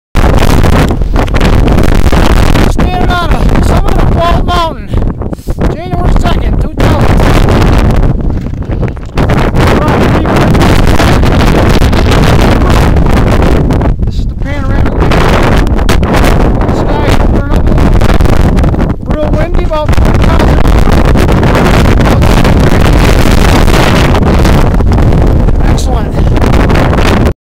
from Rocky Peak Ridge 1-2-10
This was part of the traverse form rt73 over to rt9n via Giant mnt from Roaring brook trail over Rocky Peak Ridge ,Bald Mnt and Bleuberry cobbles.This footage was from Rocky peak on the descent towards rt 9n